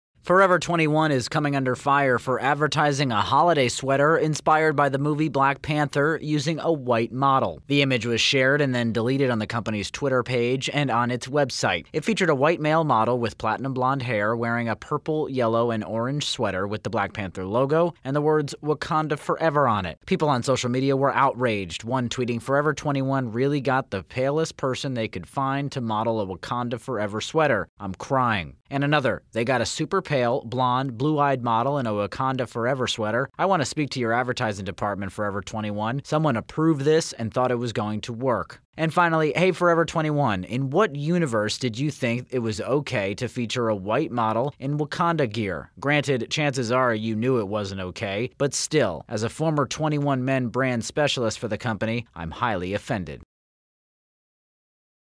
Forever 21 Facing Backlash For Featuring White Model In Black Panther Sweater | TIME
Forever 21 is coming under fire for advertising a holiday sweater inspired by the movie, “Black Panther” using a white model.